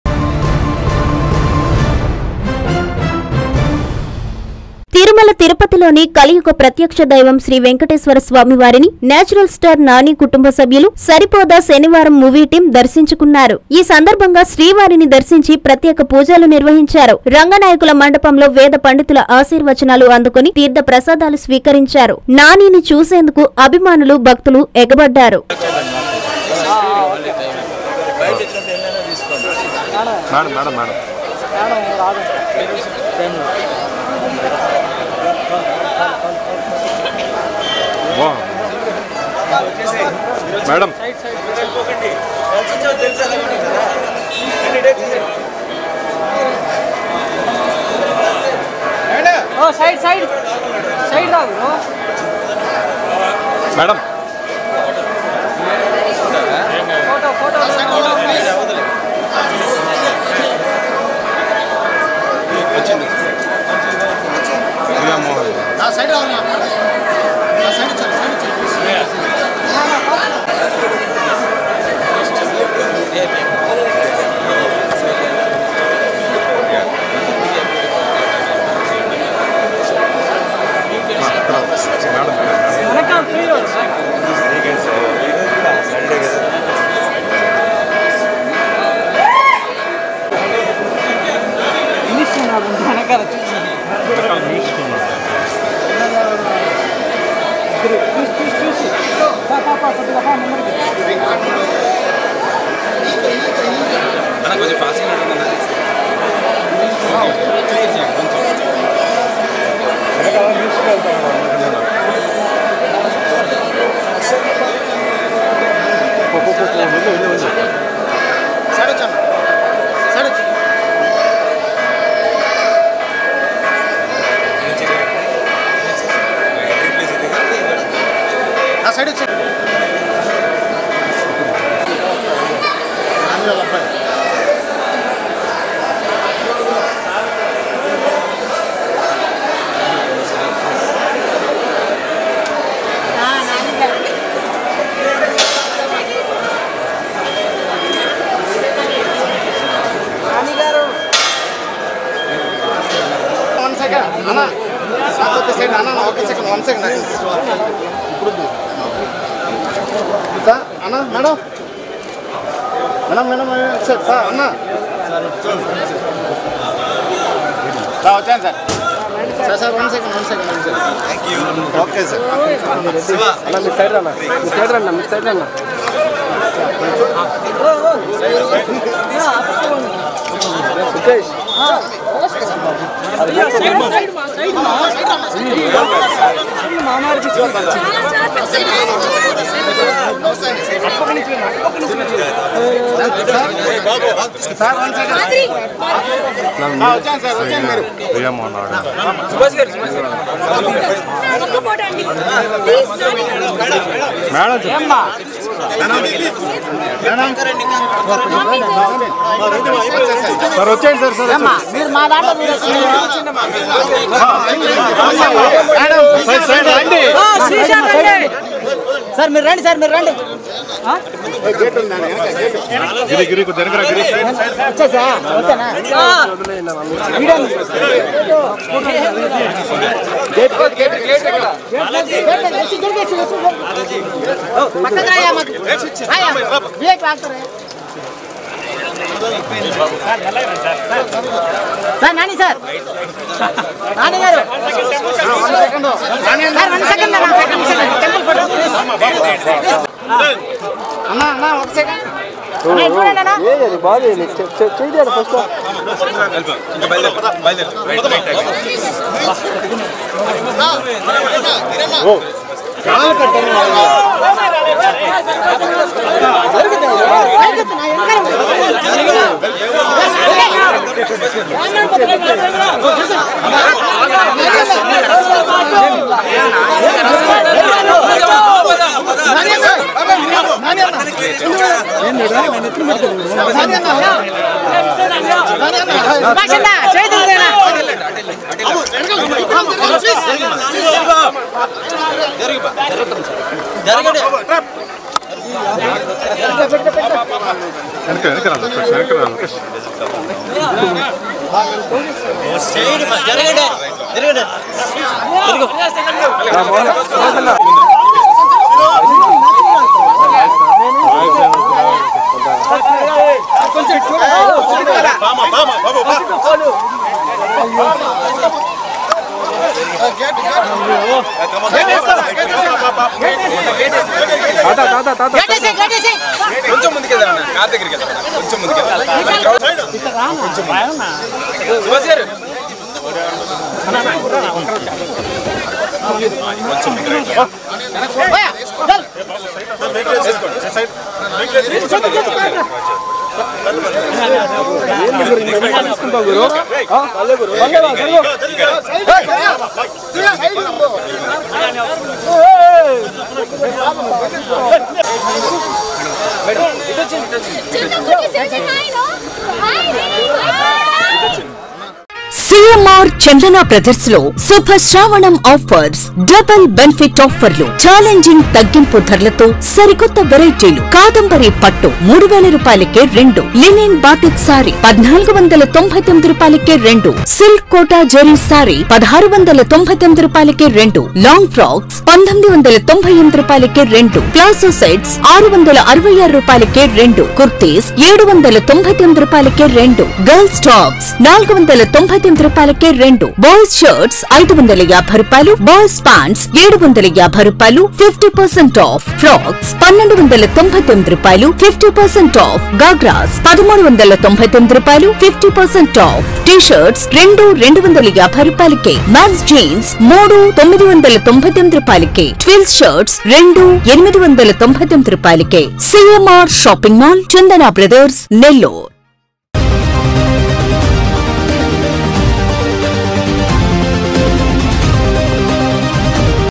శ్రీ‌వారి స‌న్నిధిలో న్యాచుర‌ల్ స్టార్‌ నాని ఫ్యామిలీ
- స‌రిపోదా శ‌నివారం మూవీ టీం